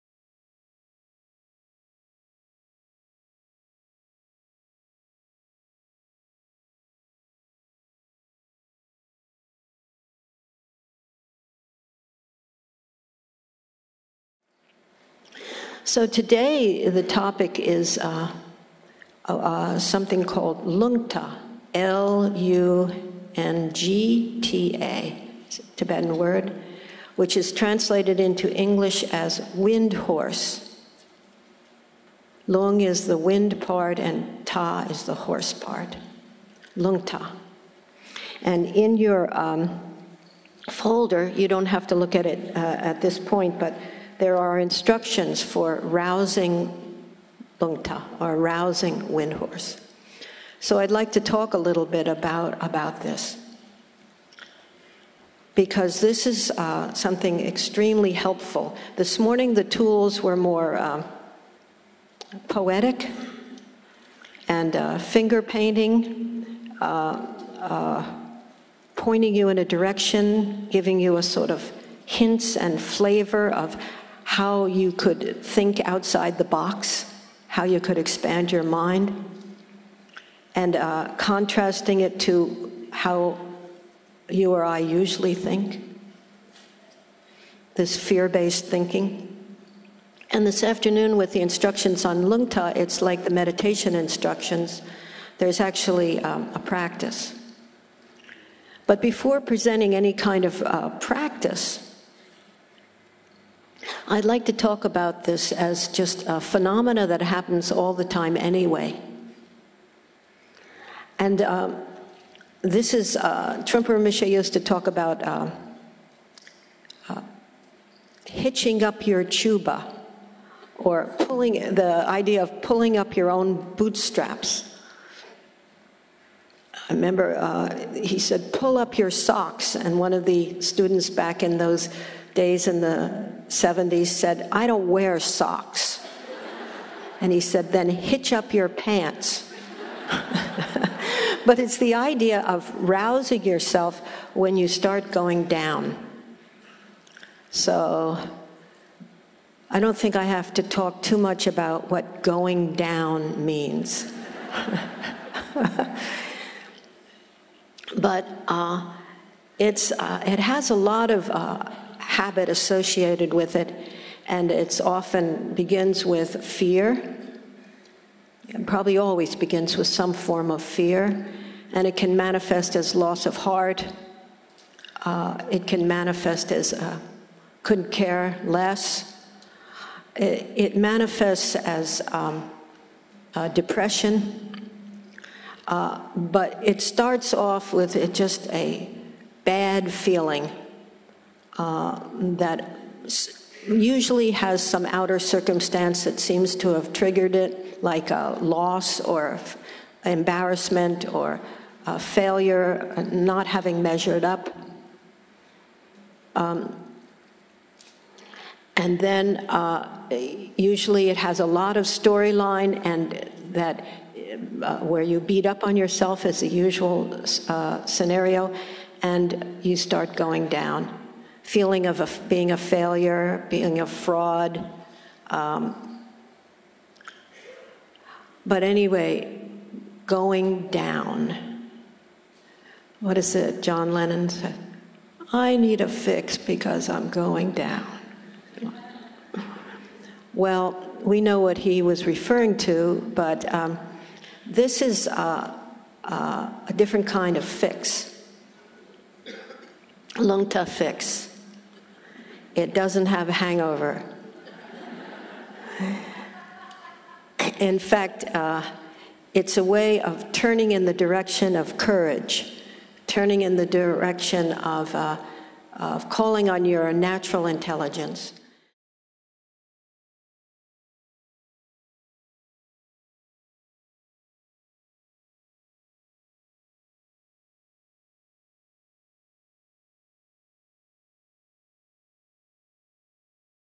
Smile at Fear with Pema Chodron
This is a recording of a retreat she gave last year to an audience of thousands in the Bay Area, inspired by the book Smile at Fear, written by her teacher Chögyam Trungpa.